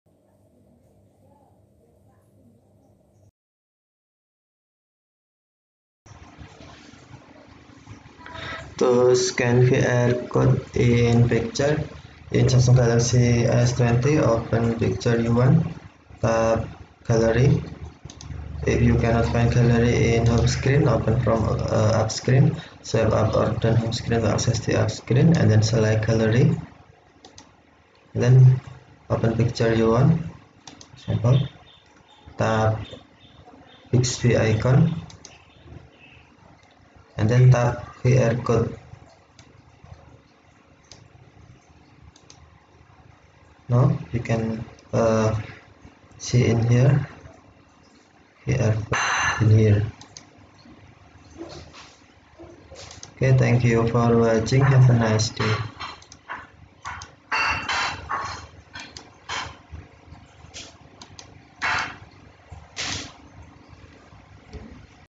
Samsung Galaxy S20 : How to scan qr code in picture (Android 11)
This video show How to scan qr code in picture in Samsung Galaxy S20.  In this tutorial I use Samsung Galaxy S20 SM-G980F/DS International version with Android 11